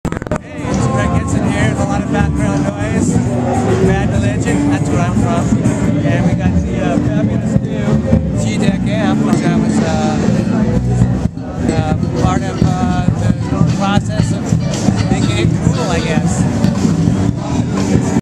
Greg Hetson | Fender
Greg Hetson talks about the G-DEC 3